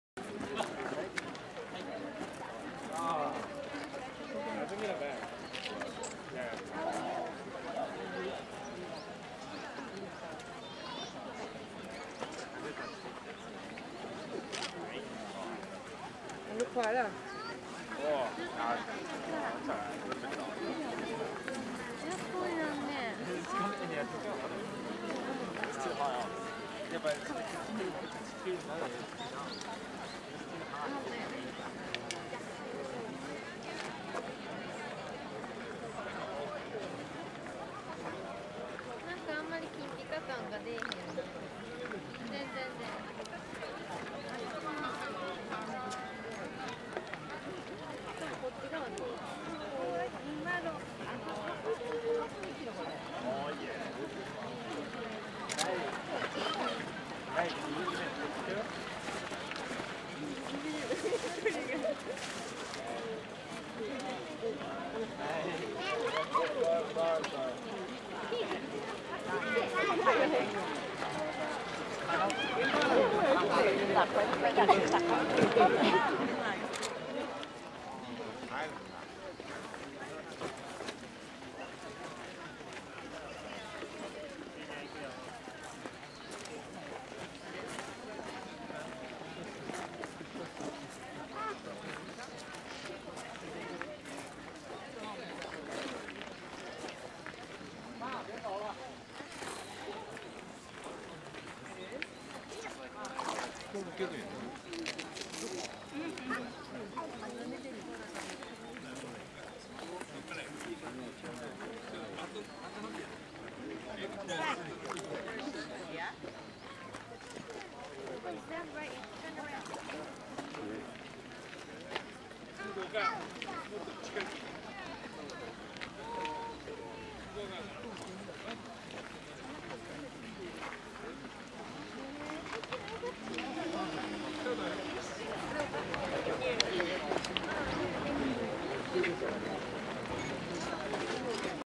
金閣寺倒影
金閣寺倒影很美 還拍到穿著和服的日本妹